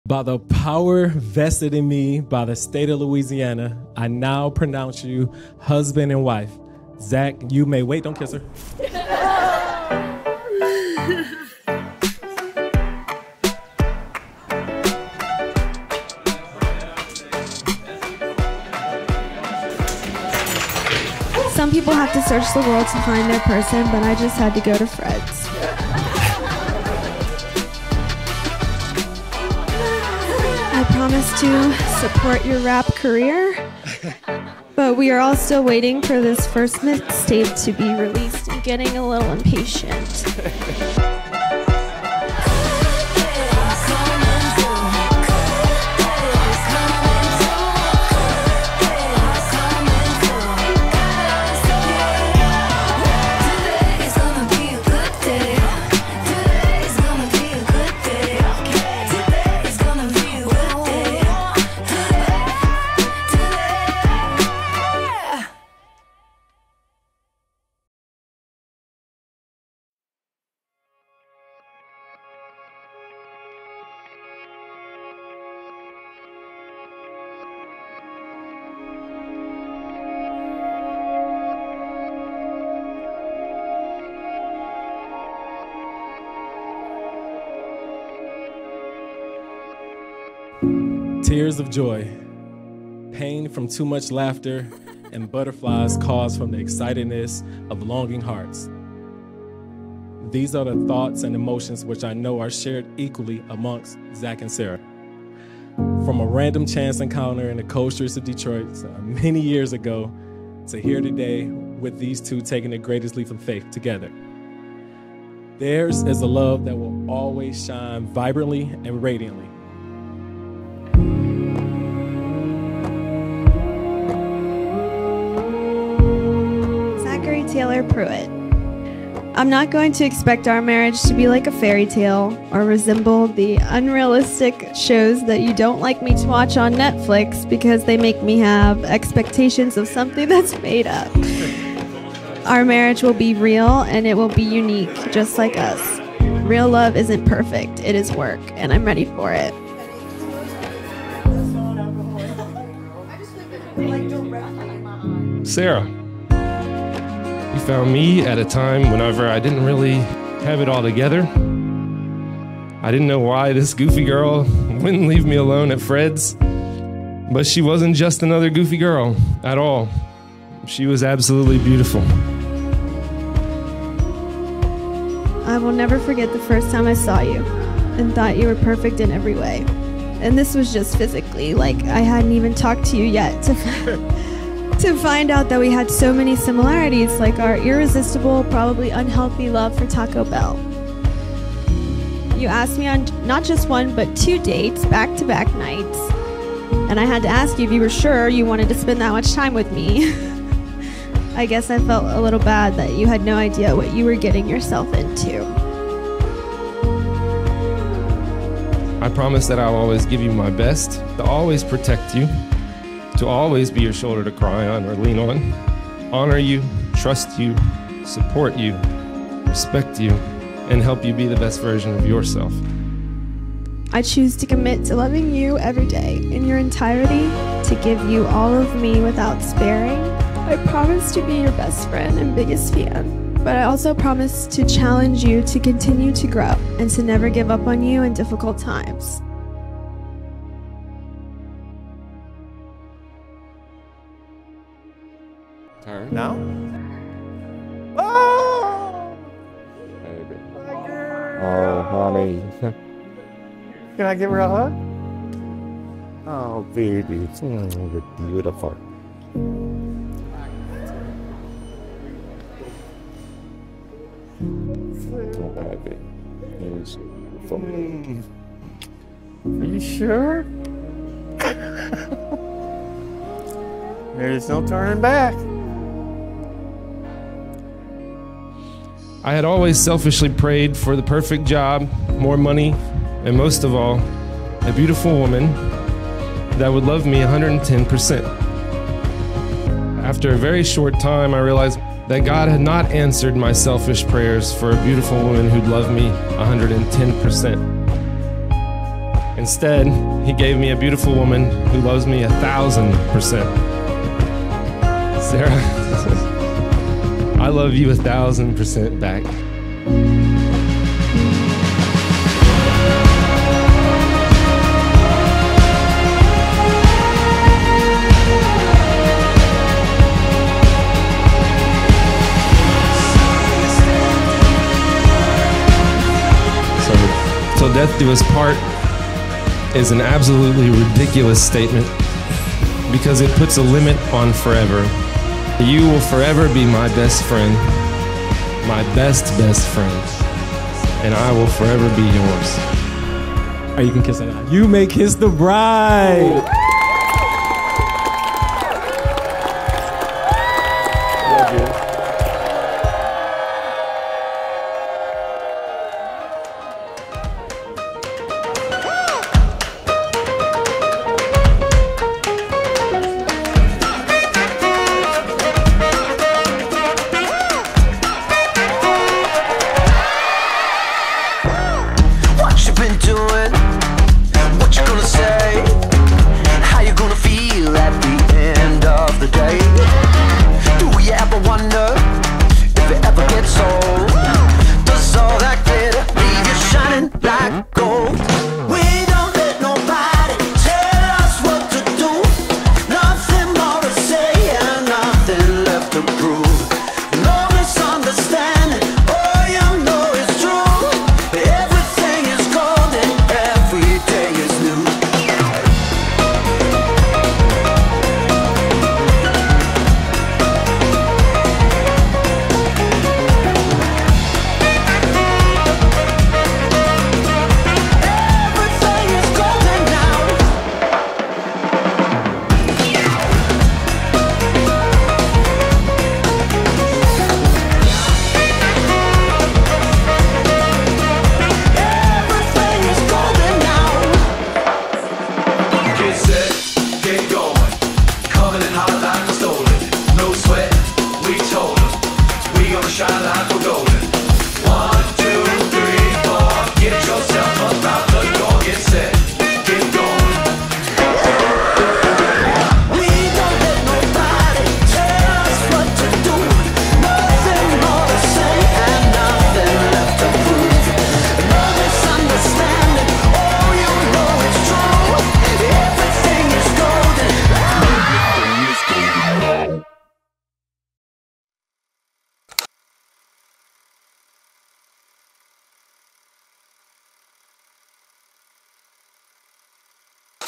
You Are My BEST BEST Friend | Fun Wedding at Shaw Center in Baton Rouge
Sarah & Zach had a fun wedding at the Shaw Center in Baton Rouge, Louisiana.

They initially met at Fred’s, a college bar near LSU.  Zach was the bouncer there, and Sarah would always hope that he was working whenever her and her friends would go out. After a few months of bothering him, he finally showed her some attention and asked Sarah to go on a couple dates.  They quickly fell in love and got married on a beautiful spring day at the Shaw Center in Baton Rouge.

I loved getting to capture every moment from their fun wedding at the Shaw Center and one of my favorite parts was getting to listen to their custom written vows and then getting to see their last dance at the very end of the night.  It was SO SWEET!  I'm so thankful for the opportunity to be a part of their beautiful wedding day!

MB01EI1PAACA6E2